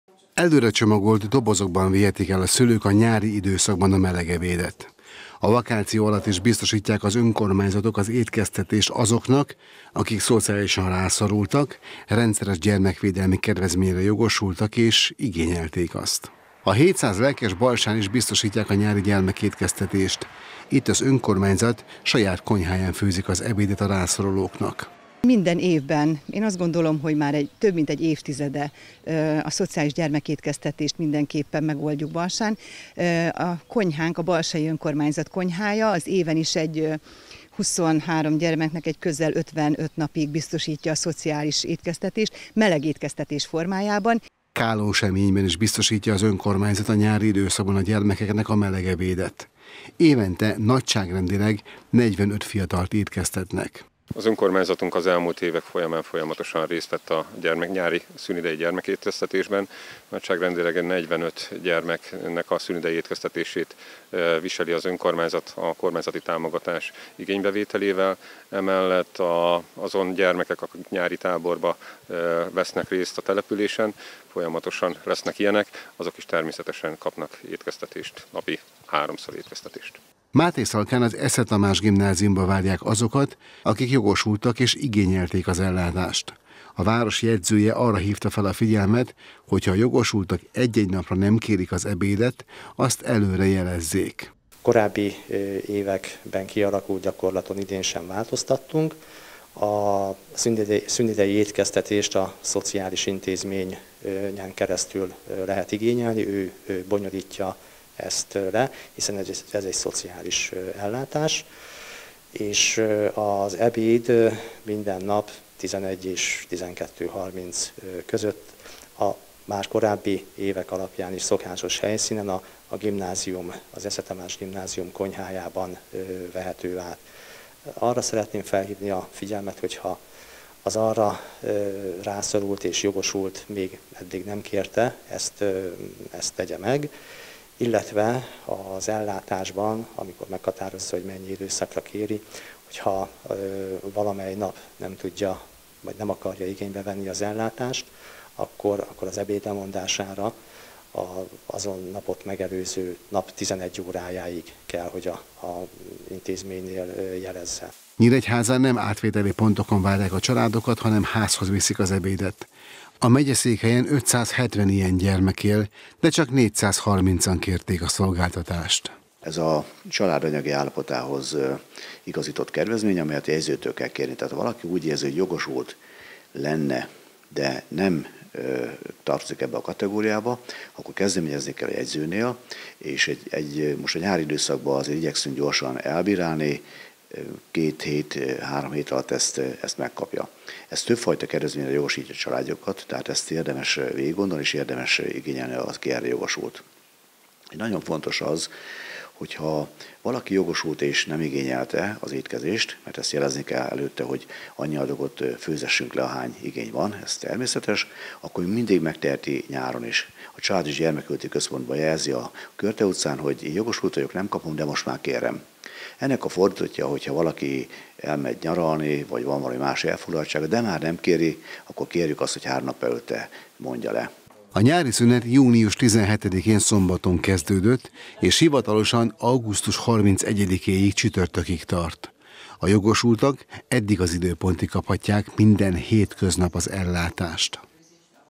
Gyermekétkeztetés a szünidőben
Idén sem maradnak meleg étel nélkül a szünidőben a rászoruló gyerekek. Az önkormányzatok azok számára biztosítják az ellátást, akik hátrányos vagy halmozottan hátrányos helyzetben élnek és rendszeres gyermekvédelmi kedvezményben részesülnek. Az ebédet igényelni kell, nem jár alanyi jogon.